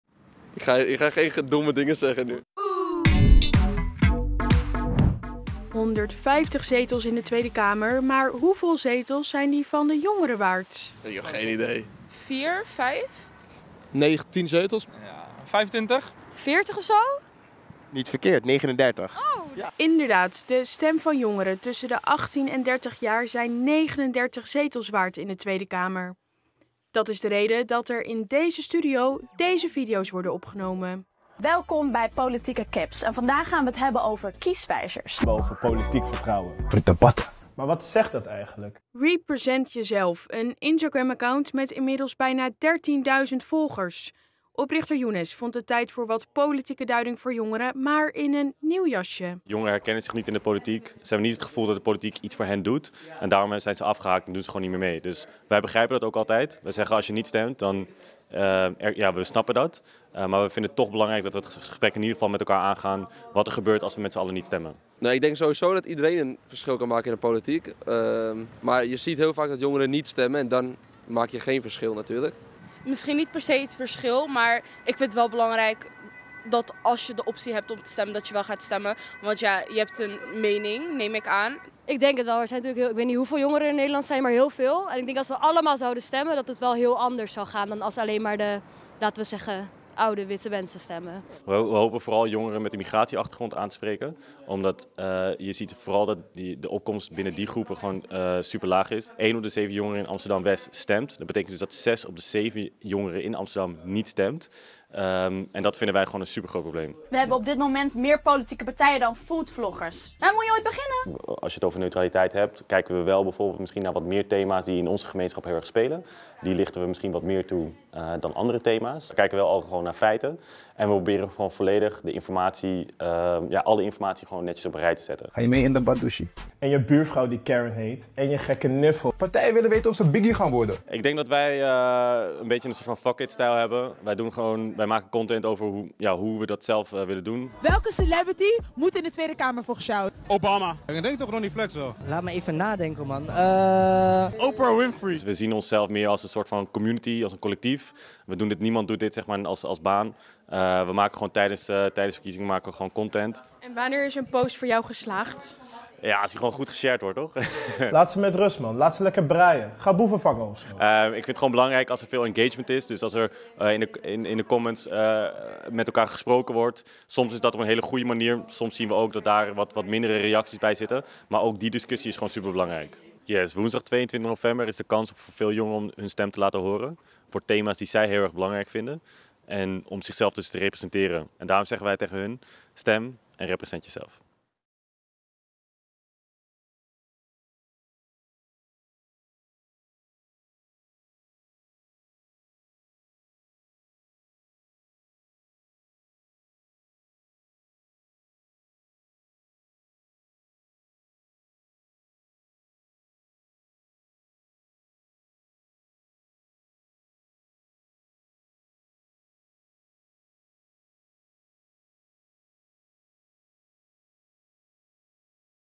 Zo helpt Instagram-account Represent Jezelf jongeren om hun politieke stem te laten horen
In aanloop naar de Tweede Kamerverkiezingen op woensdag is Younes Douari intensief bezig met politiek. Het Instagram door hem opgerichte Instagram-account Represent Jezelf heeft inmiddels bijna dertienduizend volgers. In korte video's probeert hij, samen met zijn team, jongeren uit te leggen wat in de politiek gebeurt.

Je kunt de video's zien als politieke duiding, maar dan in een nieuw jasje. En dan komen er teksten langs als "Aii, ik ben yusu" of "Partijen willen biggie worden". Represent Jezelf richt zich op jongeren met een migratieachtergrond. Dat is volgens hen de grootste groep die niet stemt.
En dat is zonde, stelt oprichter Younes: "Zo worden ze niet gerepresent in de politiek." In de keuze van de onderwerpen zie je volgens Younes ook terug dat ze zich richten op deze groep. Onderwerpen die spelen in deze gemeenschappen worden uitgelicht, zoals het woningtekort en de stijgende leefkosten. Toch is het belangrijkste onderwerp volgens Younes het conflict tussen Israël en Hamas. "Voor 60 procent van de mensen binnen onze gemeenschap is het standpunt van de partij over Palestina bepalend voor hun stem."

Als alle jongeren tussen de 18 en 30 jaar oud gaan stemmen, zijn ze goed voor zo'n 39 zetels in de Tweede Kamer: bijna een derde dus. Des te belangrijker om te stemmen, zeggen ook jongeren op straat. "Ik vind als je de optie hebt, moet je stemmen. Je hebt een mening, neem ik aan." Volgens de meeste jongeren die we spreken zou de politiek er heel anders uit zien als iedereen met stemrecht zou stemmen.

AT5 Extra
Meer video's van AT5.